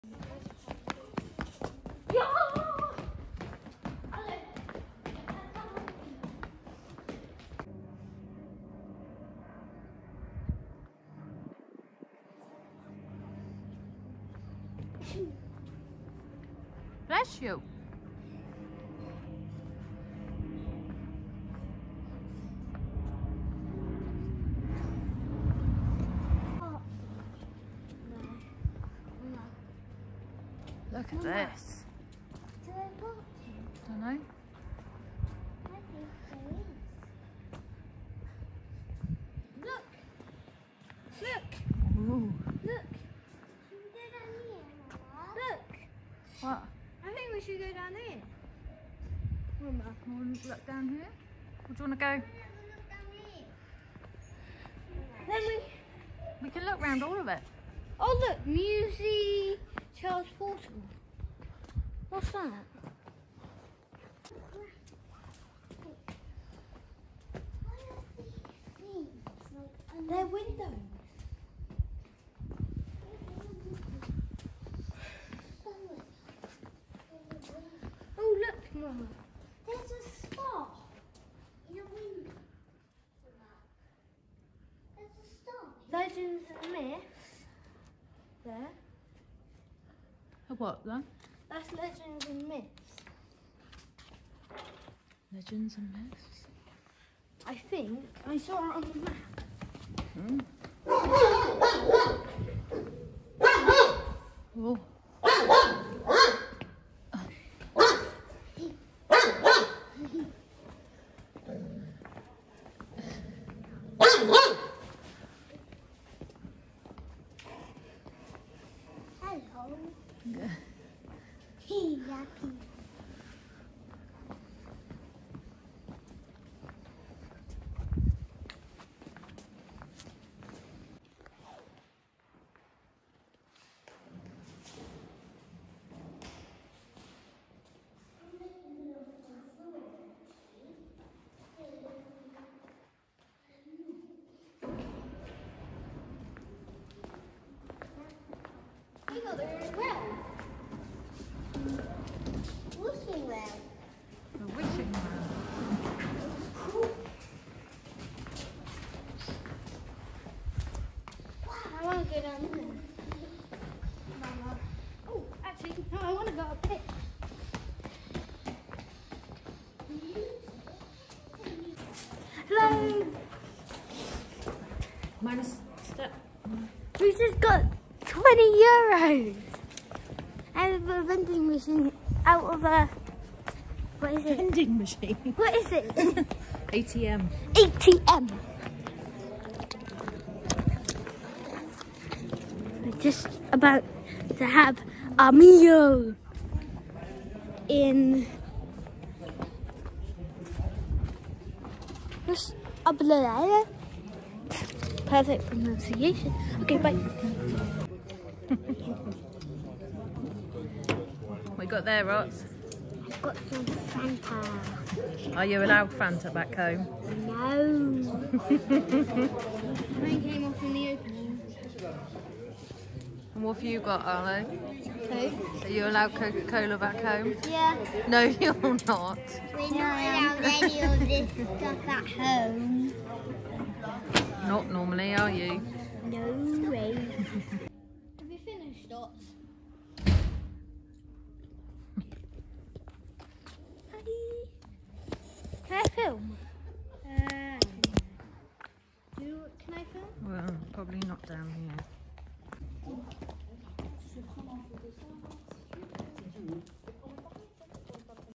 Visit to Halles de Cordes - 🇫🇷 South of France with kids - Why do boys like to race everywhere??
We visited Halles de Cordes-sur-Ciel 🇫🇷☀, a medieval town in the south of France for the day. With the usual drinks and a bit of food included. We love to take time to travel to lovely little villages. This one happens to be a medieval village near to our house sit.